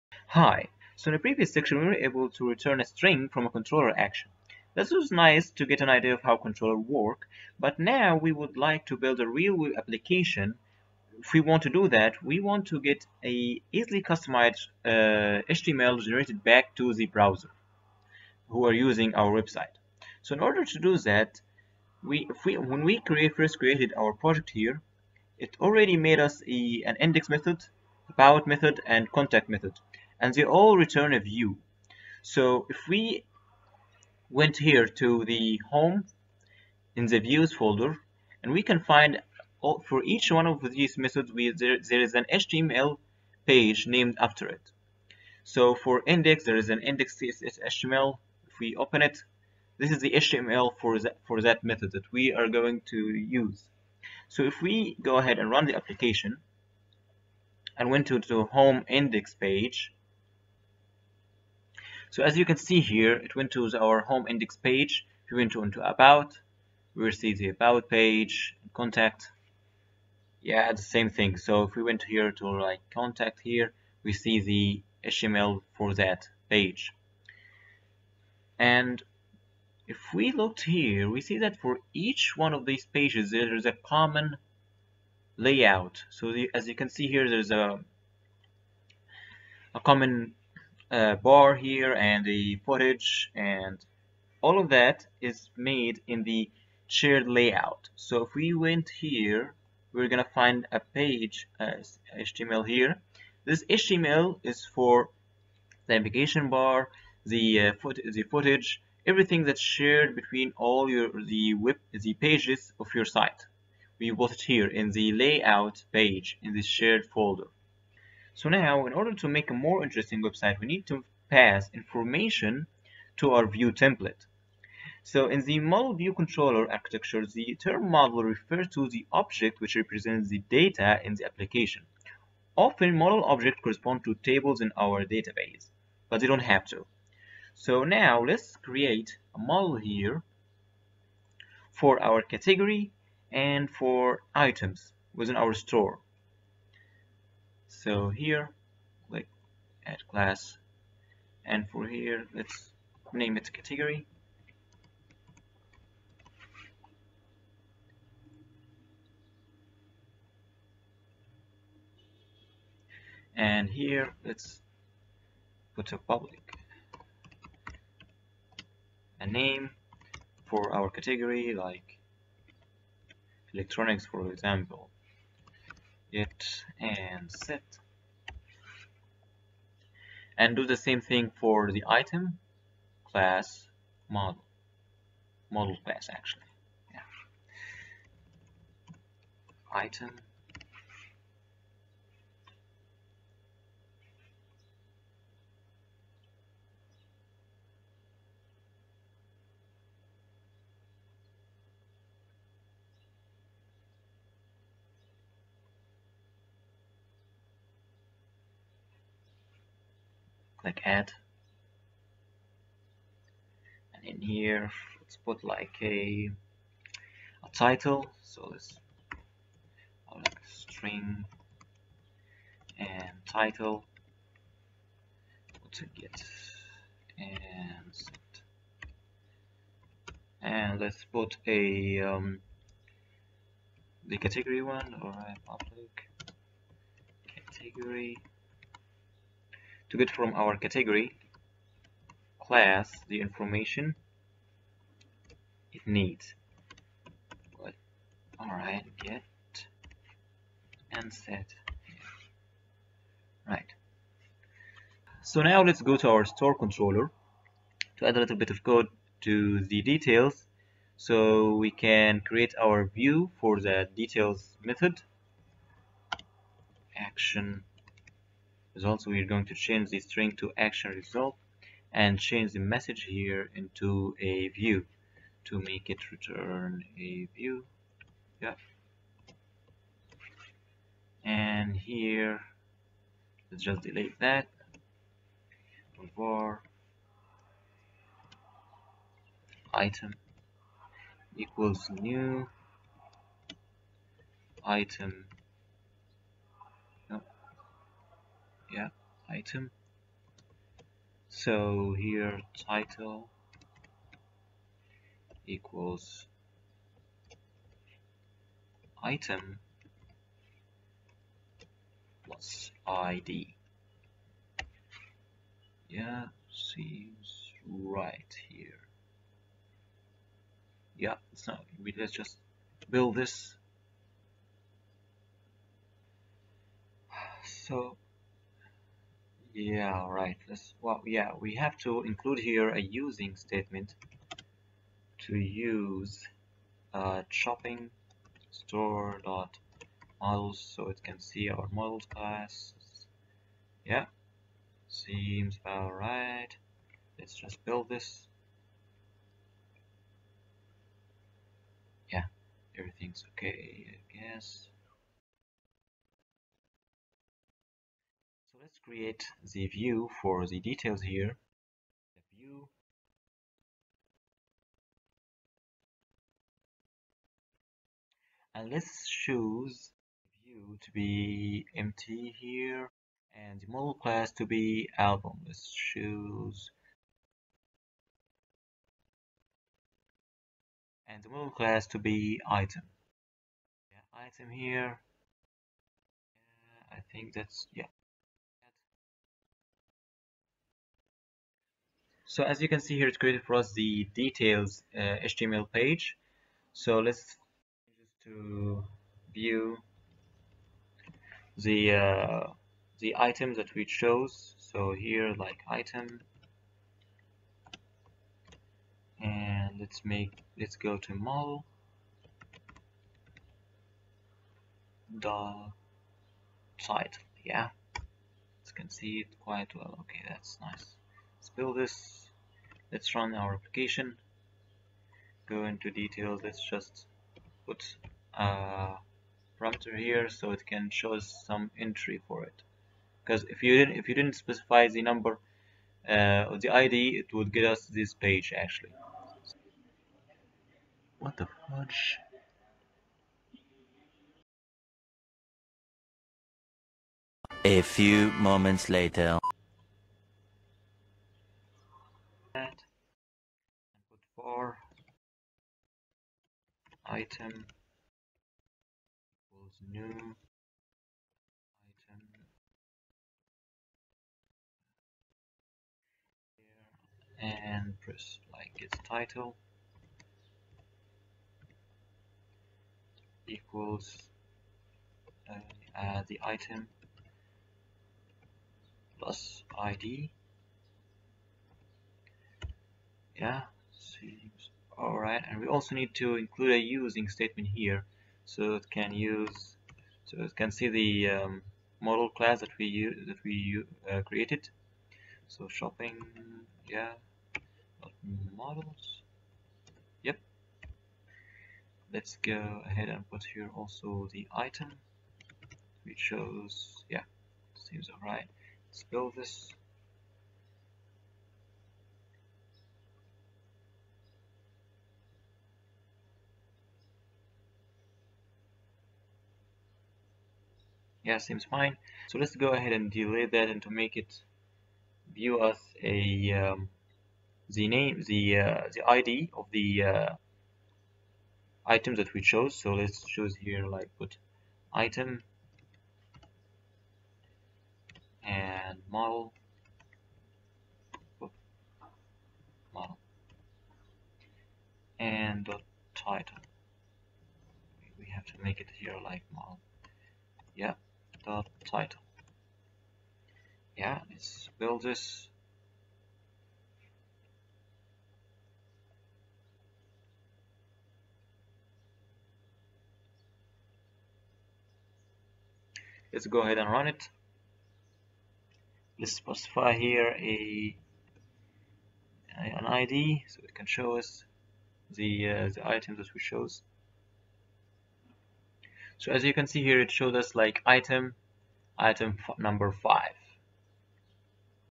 ASP.NET MVC Views and Models part 1 | Online Shopping Store
Get a grab on how to use Models and Link them with the appropriate views ASP.NET MVC